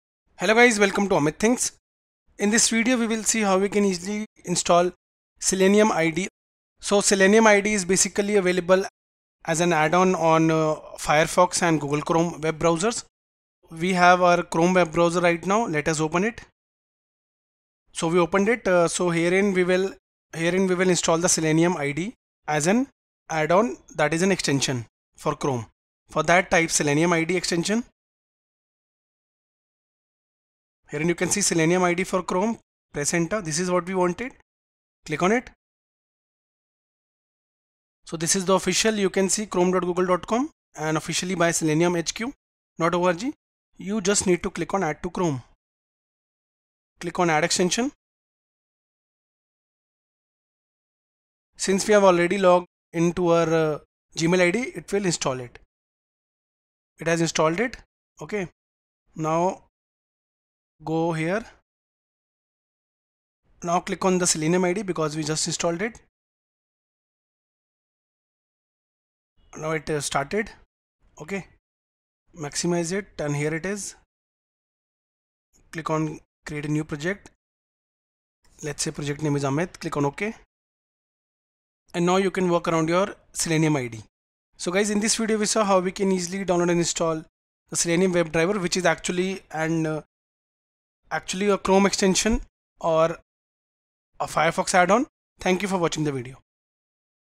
How to Install Selenium IDE Chrome Extension
In this video, learn how to install Selenium IDE. It can be installed as a Chrome extension or Firefox add-on. Selenium IDE is an integrated development environment for Selenium tests. You can record, edit, and debug tests with Selenium.